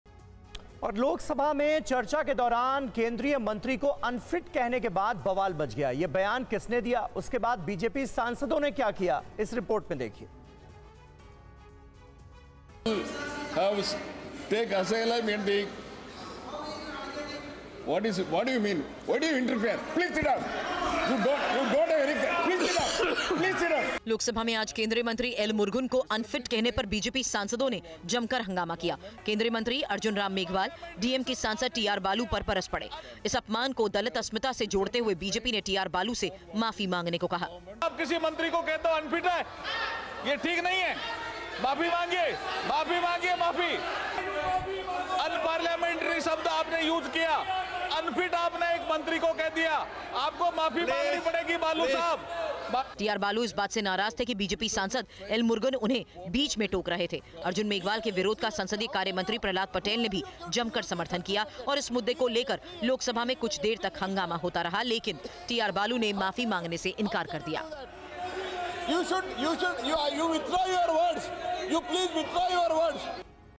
Parliament Session: लोकसभा में चर्चा के दौरान Union Minister को अनफिट कहने के बाद बवाल मच गया
लोकसभा में चर्चा के दौरान Union Minister को TR balu ने अनफिट कहने के बाद बवाल मच गया... ये बयान किसने दिया...उसके  बाद BJP सांसदों ने क्या किया देखिए ये रिपोर्ट